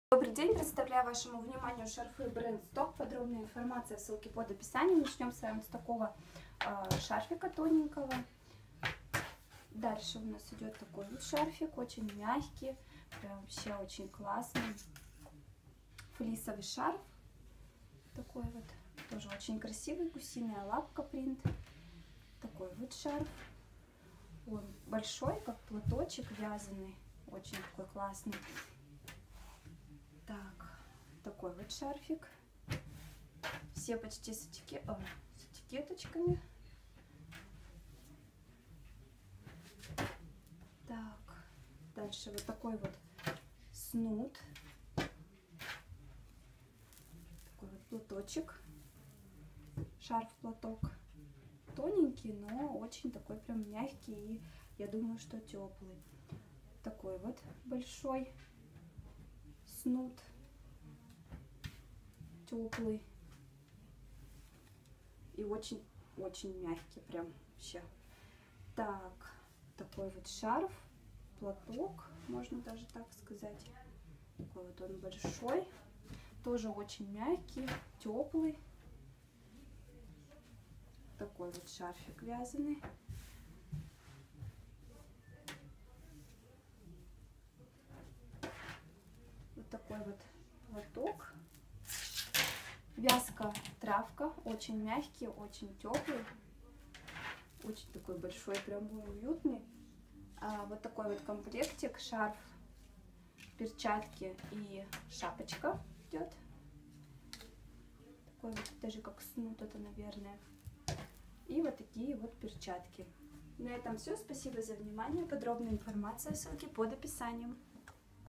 ❄-10% СКИДКА❄сти122ш. Шарфы Brand сток
⛔Акция действует до 31.01.24
сти122ш. Шарфы Brand сток
Упаковка 2,6 кг.
Цена 1349 руб/кг.(с учетом скидки)